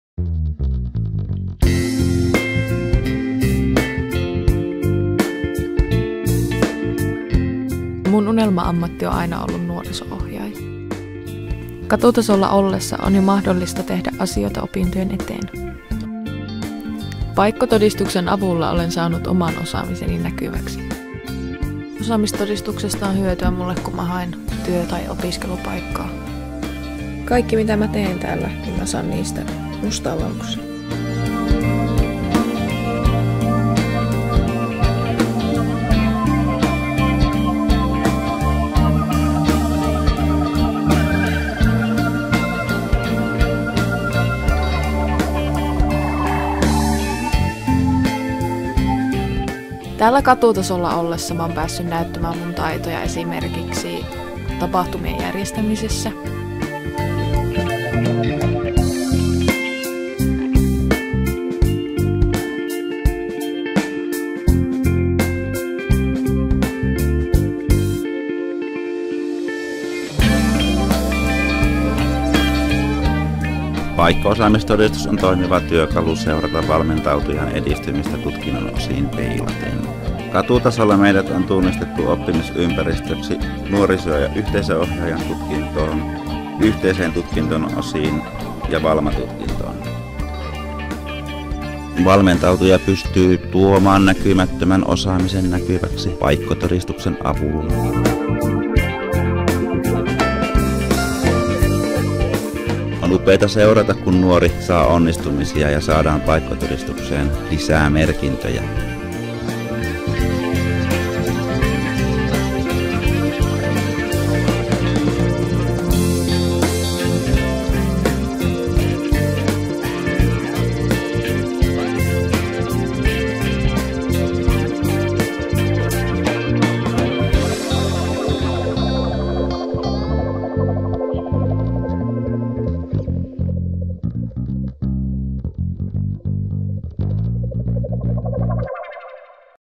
Näkymätön osaaminen näkyväksi PAIKKO-osaamistodistuksella
Myös Jyväskylän Katulähetyksen Katutasolla kirjoitetaan PAIKKO-osaamistodistuksia, joiden avulla osaaminen ja onnistumiset tehdään näkyväksi.